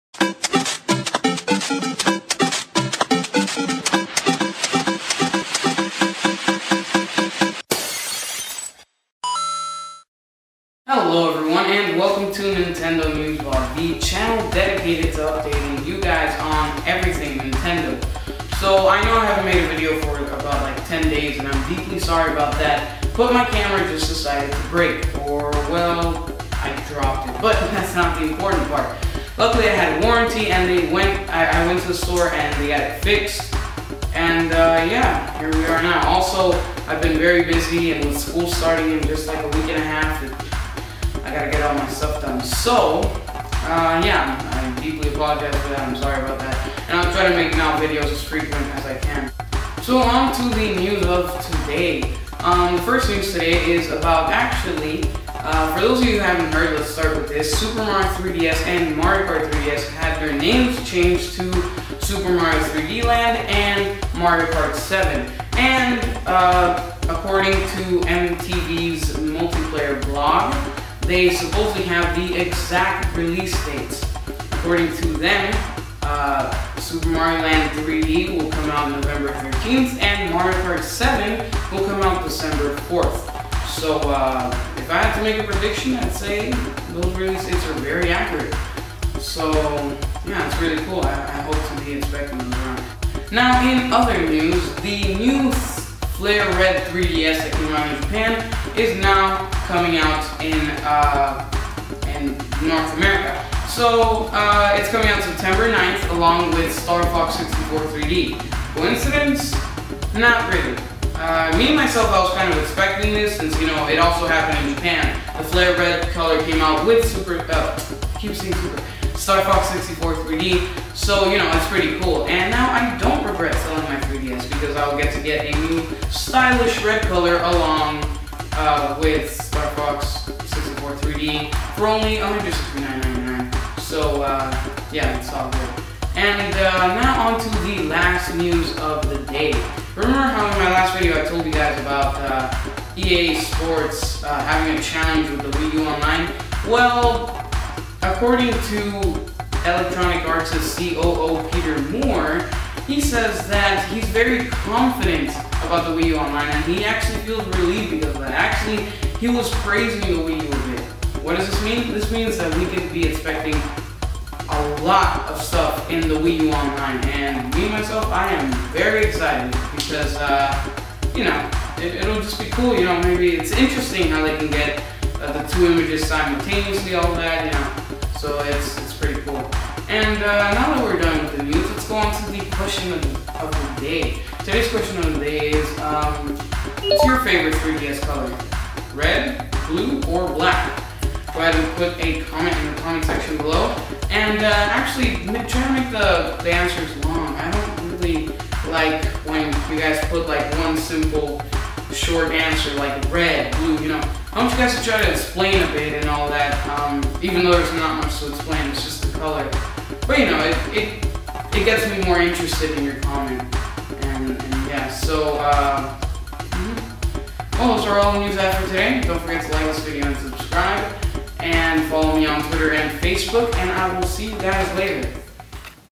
Super Mario 3D Land and Mario Kart 7 Release Dates, Flame Red 3DS UPDATE, EA on Wii U Online
Super Mario 3D Land and Mario Kart 7 have finally their very own release dates!!
Super Mario 3D Land: November 13
Mario Kart 7: December 4th
Flame Red 3DS is coming out in North America on September 9 along with Starfox 64 3D, and EA is confident about the Wii U's online!